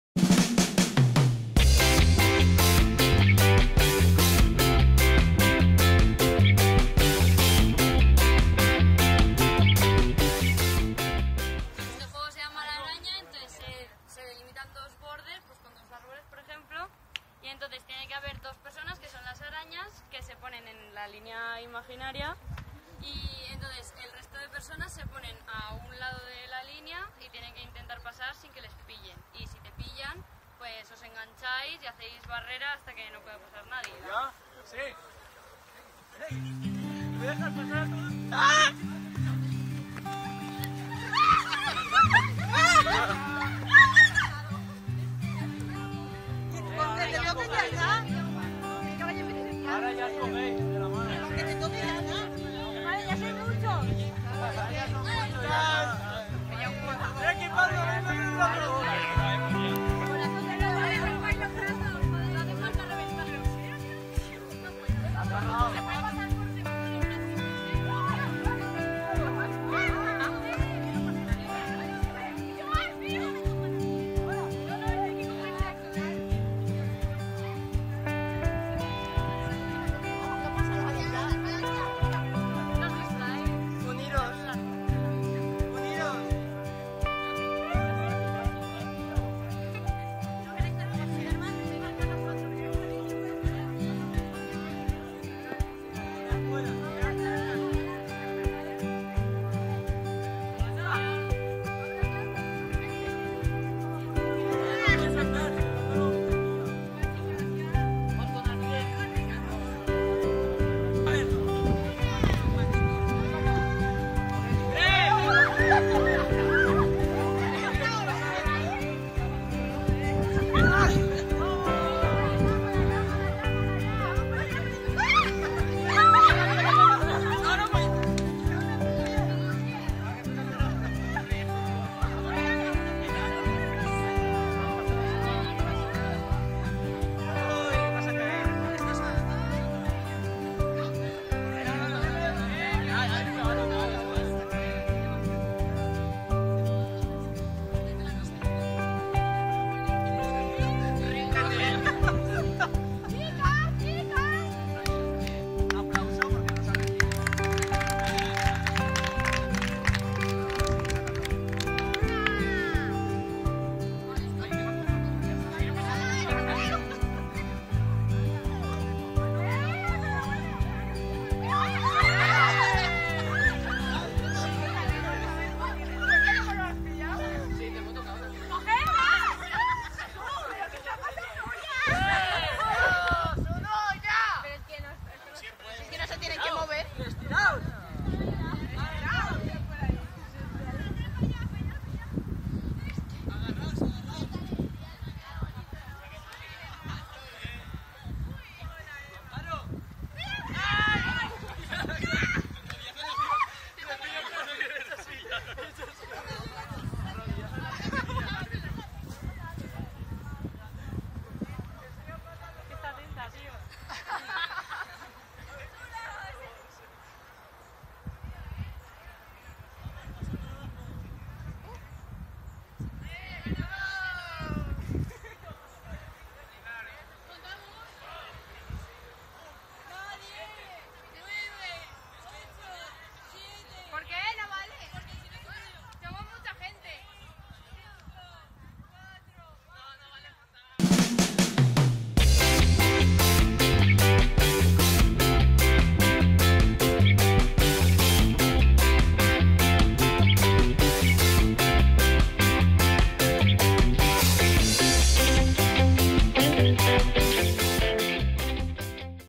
Juego 𝗟𝗔 𝗔𝗥𝗔Ñ𝗔. Coger | Correr | Trabajo en Equipo | Dinámica de Grupo | Campamento
𝗟𝗔 𝗔𝗥𝗔Ñ𝗔 ⁽ᴹⁱʳᵃ ˡᵒˢ ᵈᵉᵗᵃˡˡᵉˢ⁾ 👇🏻
Tendremos que pasar al otro lado de la línea sin que nos pillen. Los jugadores cruzan una línea evitando que las "arañas" los toquen; si son atrapados, se unen a la cadena de arañas.
🔹𝘗𝘳𝘰𝘱𝘰𝘯𝘦_ Trabajo en equipo, coordinación, sensación de riesgo,  esfuerzo físico, destreza, atención.

Cᴼᴹᴾᴿᴬ ᴇʟ ᴺᵁᴱⱽᴼ 𝗠𝗮𝗻𝘂𝗮𝗹
✨𝟏𝟎𝟎 𝐉𝐔𝐄𝐆𝐎𝐒 𝐝𝐞 𝐆𝐫𝐮𝐩𝐨✨
 𝘋𝘦𝘴𝘤𝘳𝘪𝘱𝘤𝘪ó𝘯 𝘦𝘴𝘤𝘳𝘪𝘵𝘢 𝘺 𝘝𝘪𝘥𝘦𝘰𝘴

𝗟𝗔 𝗔𝗥𝗔Ñ𝗔 👇🏻
Necesitaremos un espacio amplio y despejado con una línea recta dibujada en el suelo situada en en centro del terreno de juego.
🕷️ Este juego requiere de dos voluntarios o voluntarias que harán el papel de arañas, colocándose en una línea recta de la cuál no podrán salir, pero sí moverse a lo largo de ella.
🏃‍♂️ El resto del grupo estará a un costado de la línea esperando la señal para poder pasar al otro lado, intentando que las arañas no los pillen (con que las arañas le toquen es suficiente). Los jugadores tienen un tiempo limitado para cruzar, y si no lo logran antes de que el tiempo termine, también se convierten en arañas.
🕸️ En caso de que la araña pille a un participante, este pasará a ser una araña más, cogiéndose siempre de las manos para ir formando una cadena a la que se unirá cada nuevo participante que las arañas atrapen.
 El juego termina cuando todos los jugadores han sido atrapados y se han convertido en arañas.

🔹𝘍𝘰𝘳𝘮𝘢𝘤𝘪ó𝘯_ Curso Monitor/a a de Tiempo Libre Educativo
🔹𝘊𝘶𝘳𝘴𝘰_ Valencia (Valencia, España) 2019.
🔹𝘗𝘳𝘰𝘧𝘦𝘴𝘰𝘳_ Ignacio Martín García.